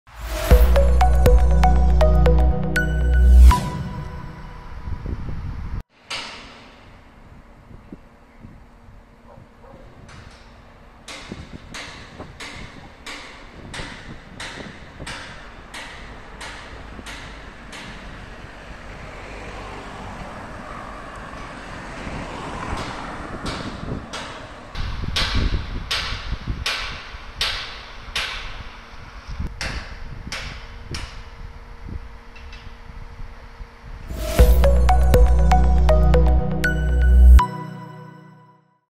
Ko to noću gradi po Dorćolu?
Radovi u kasnim noćnim satima poslednjih par meseci, i to jednom do dva puta nedeljno, obavljaju se na gradilištu koje obuhvata ulice Cara Dušana, Knićaninovu i Skender Begovu na Dorćolu.

Prijave nadležnim inspekcijama i komunalnoj policiji nisu urodile plodom.